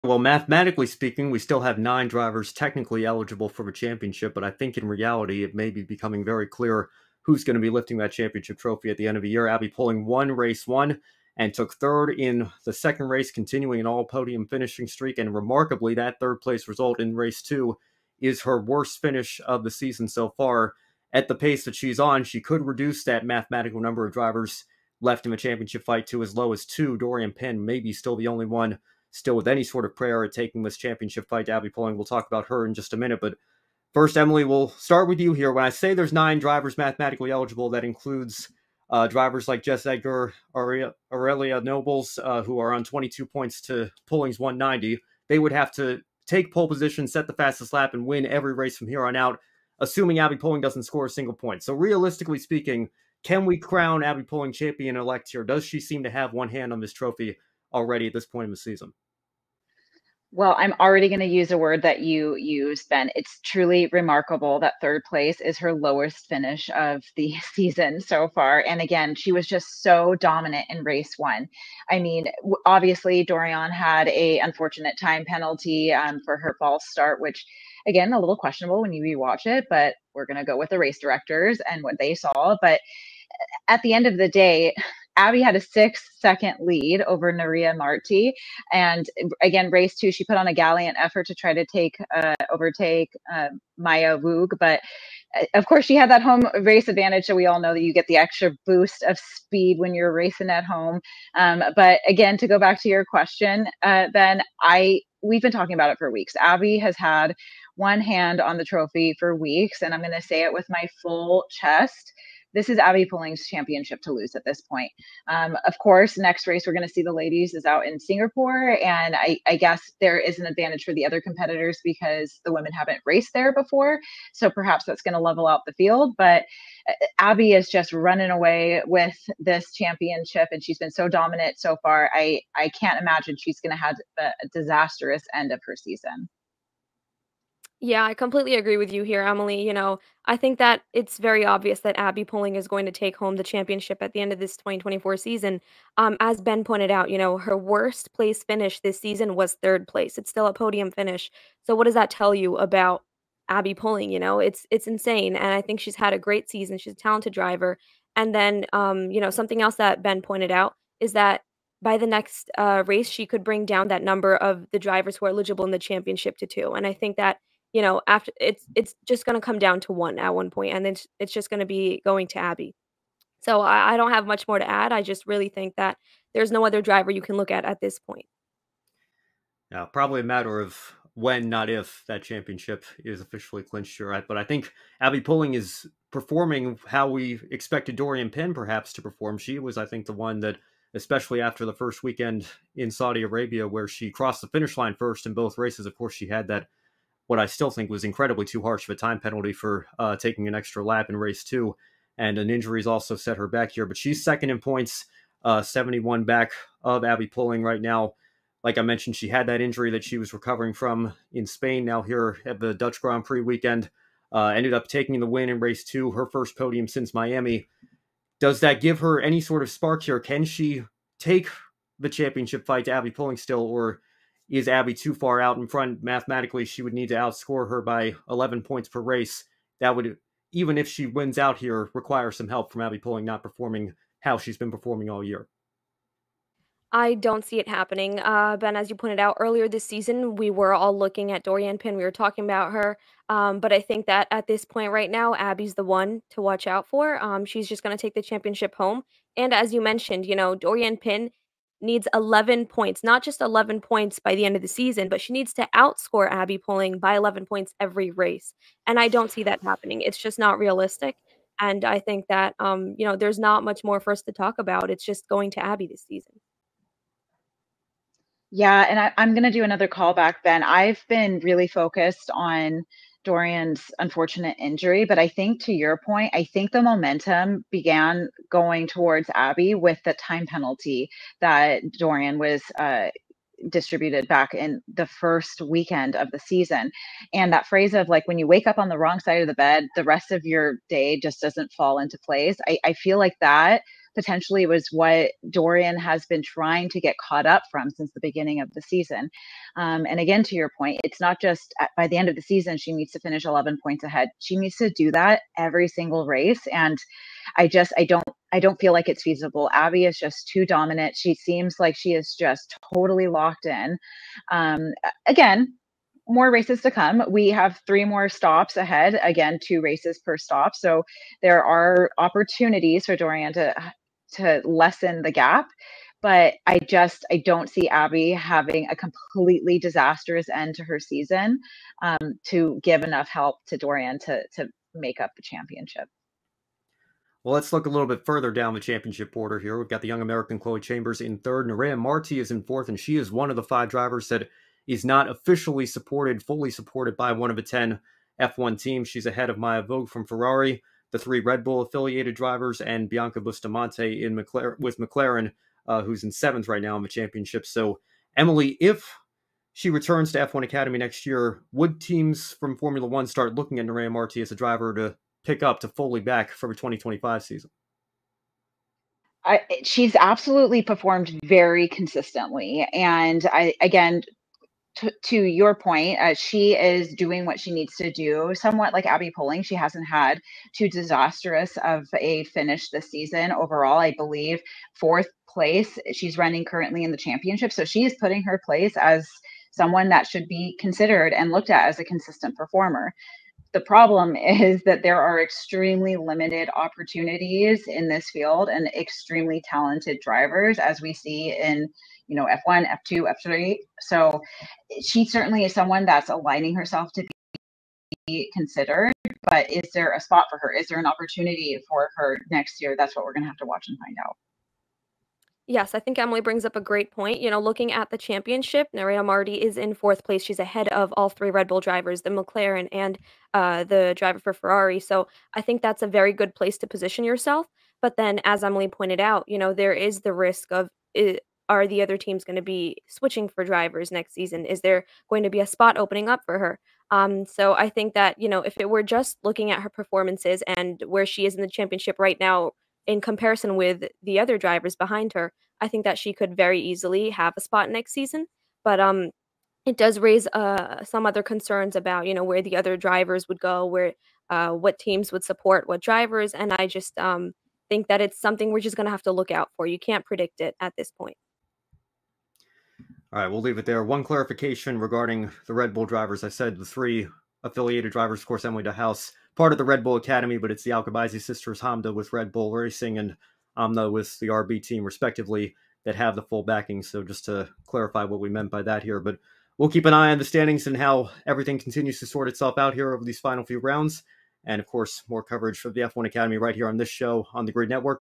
Has Abbi Pulling all but clinched the F1 Academy title? | GRID Women In Motorsport
On Women In Motorsport, Ben Schneider, Emily Sharp, and Esha Azeem react to the latest F1 Academy round at Zandvoort and discuss whether Abbi Pulling's championship lead is now insurmountable even for her closest championship competitor, Doriane Pin.